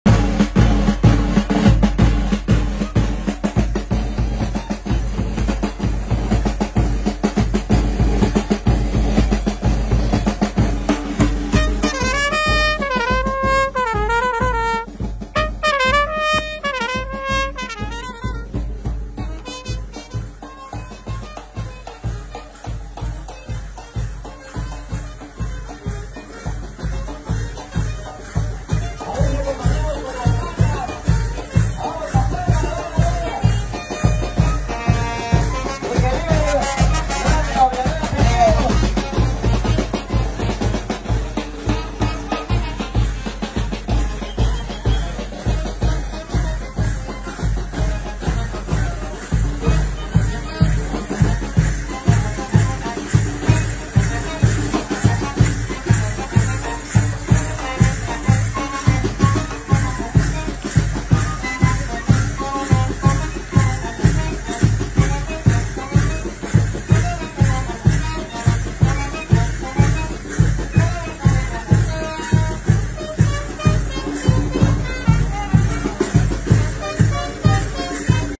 Diablicos en San Ignacio rinden homenaje a niño Jesús
SanIgnacio | 27 y 28 de diciembre se realiza recorrido de los “Diablicos” en San Ignacio rindiendo homenaje al niño Jesús.